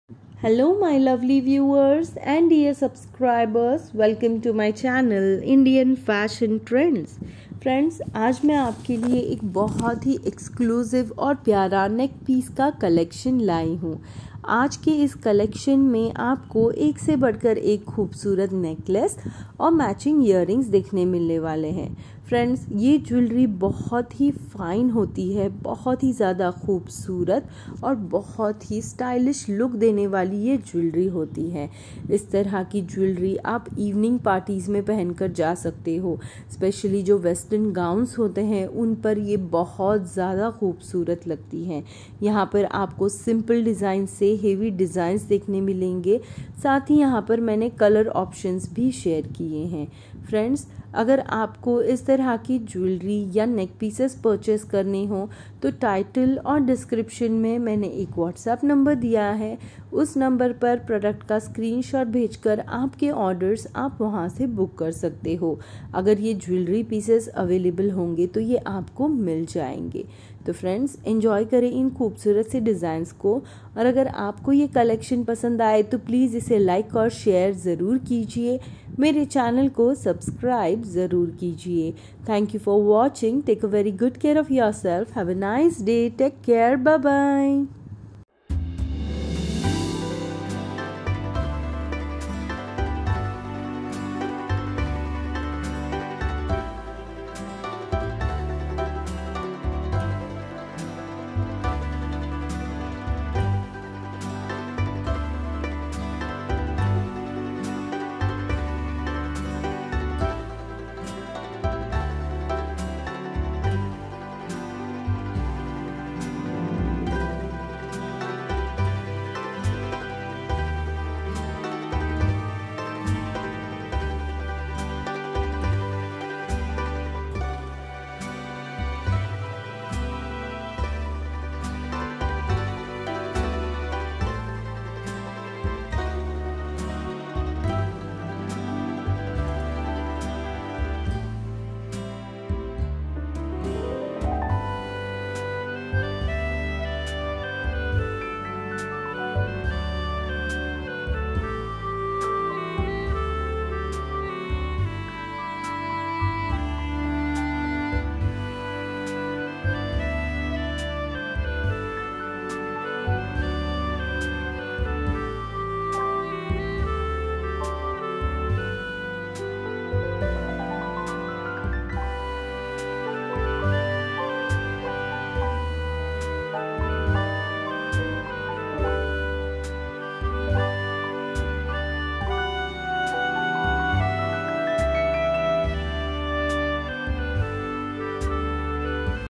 Simple Necklace Set Design for woman / AD Jewellery set - Indian Fashion Trends
Here in this video, I have shared some latest Simple AD Necklace Set Design for Women.